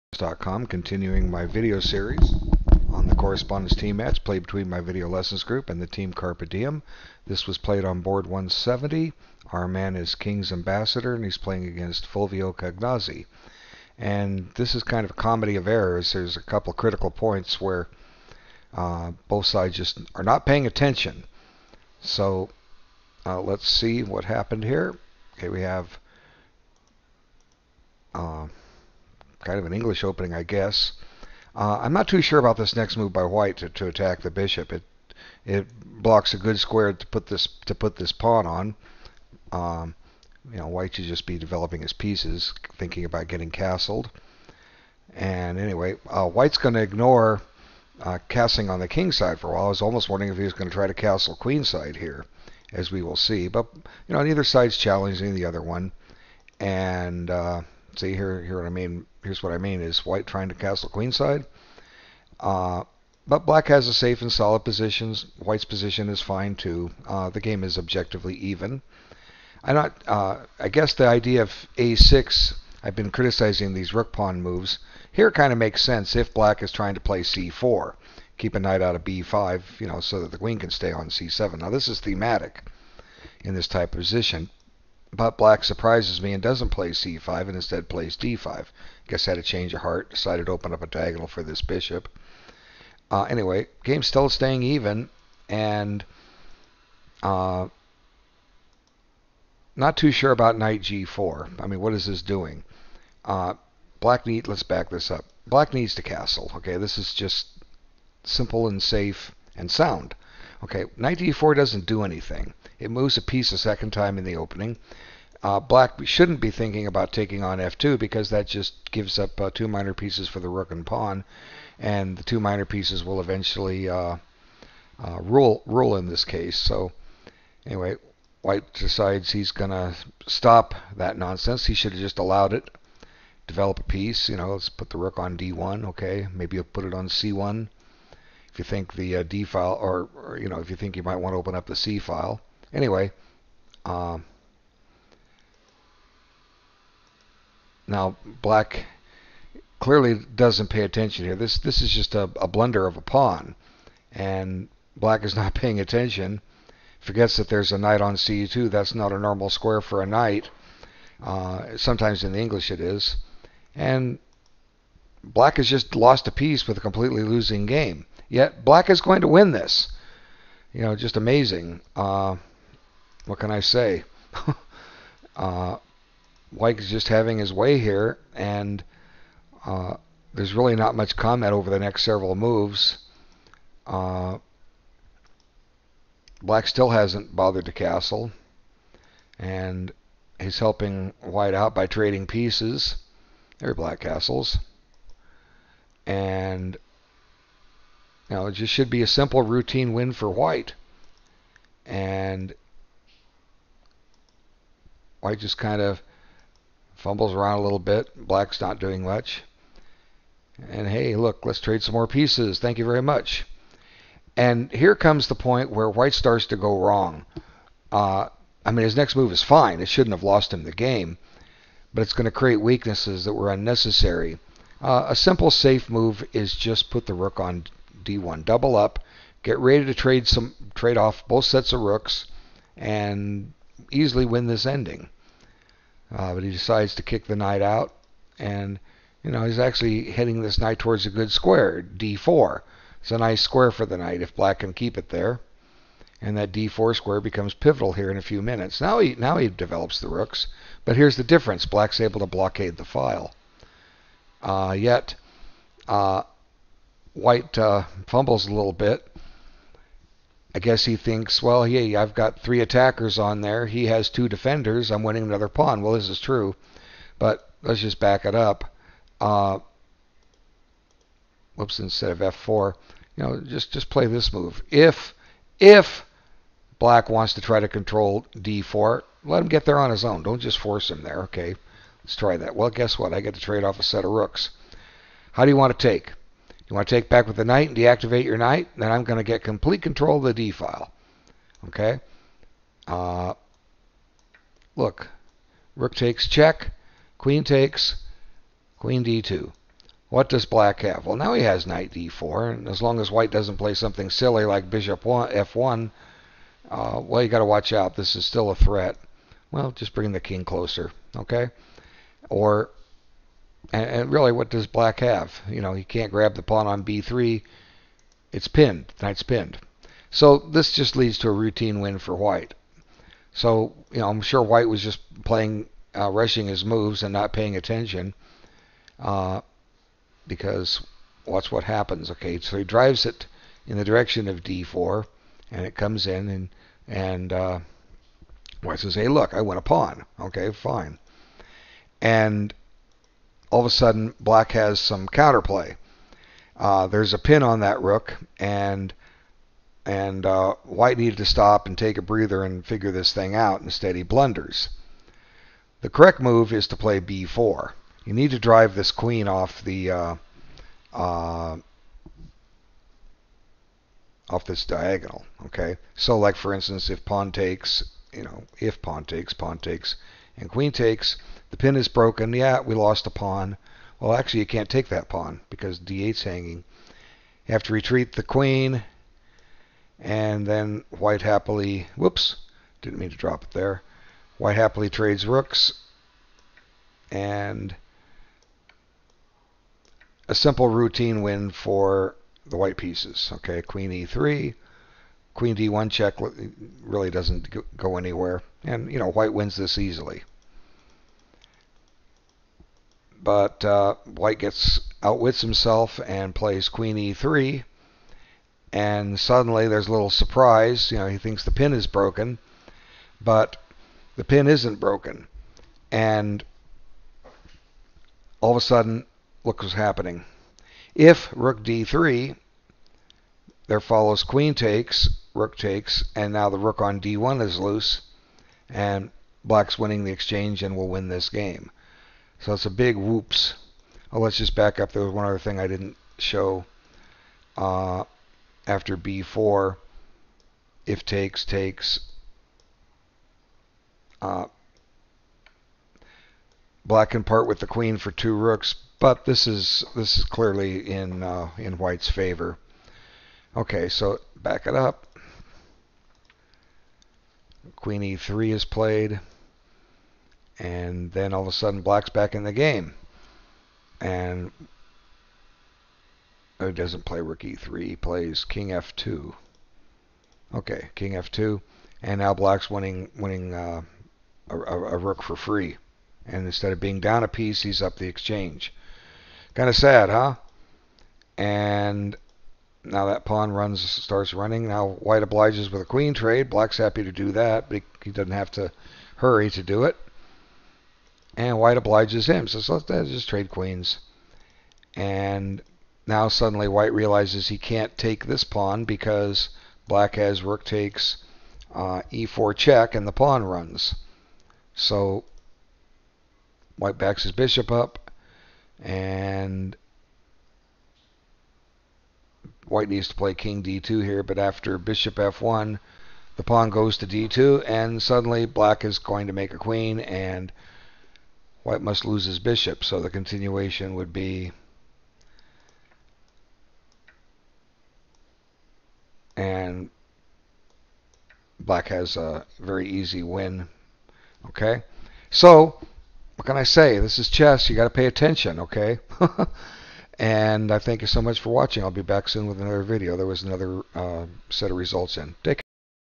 TeamMatch143
[Event "Free War Land of CARPE DIEM -- Open -- 3 days/move - Board 170"]
[Site "Chess.com"]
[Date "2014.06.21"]
[White "FulvioCagnazzi"]
[Black "Kingsambassador"]
[Result "0-1"]
[WhiteElo "1443"]
[BlackElo "1702"]
[TimeControl "1 in 3 days"]
[Termination "Kingsambassador won by resignation"]

1.c4 c6 2.Nc3 d6 3.Nf3 Bf5 4.Nd4 Bg6 5.d3 e6 6.b3 Nd7 7.Bb2 Ngf6 8.Qd2 Qc7 9.g3 a6 10.Bg2 d5
 11.O-O Bc5 12.Nc2 Ng4 13.e3 d4 14.exd4 Bxd4 15.Nxd4 Rd8 16.h3 Nge5 17.Ne4 c5 18.Nf3 f6 19.Bc3 Bxe4 20.dxe4 O-O
 21.Qe2 Ng6 22.Rfd1 Rfe8 23.a3 a5 24.Qe1 b6 25.Rd2 Nde5 26.Nxe5 Nxe5 27.f4 Nc6 28.Rad1 Nd4 29.Bxd4 cxd4 30.Rxd4 Qc5
 31.Qe3 e5 32.Kf2 Rxd4 33.Rxd4 exd4 34.Qc1 d3+ 35.Qe3 f5 36.Qxc5 bxc5 37.Ke3 fxe4 38.g4 Rd8 39.Bf1 d2 0-1